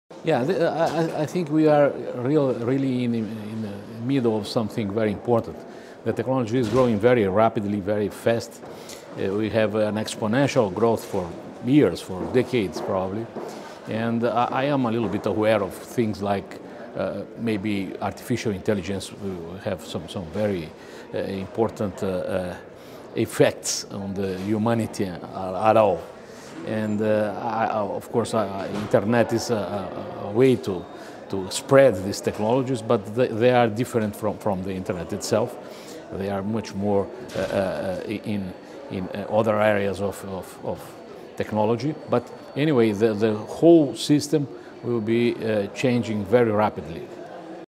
Global IGF 2018: Demi Getschko on Best New Digital Development
Demi Getschko, CEO, NIC.br, Brazil, shares their answer to the question, “What new digital technologies will create the best opportunities for a better world all over the next 10 years?” Recorded at the Global Internet Governance Forum 2018 in Paris, France, by the Imagining the Internet Center, an initiative of Elon University, North Carolina, USA.